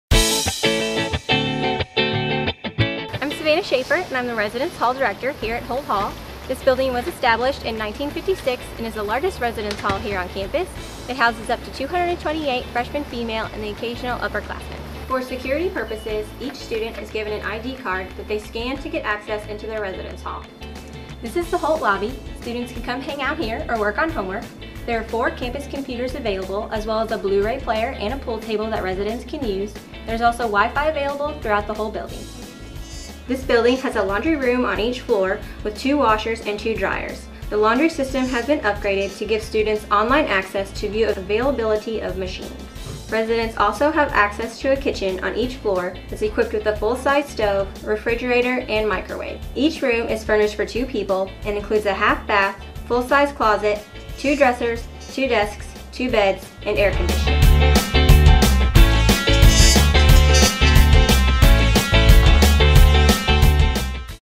Holt Hall
A quick look at Holt Hall, the female residence hall at Central Methodist University.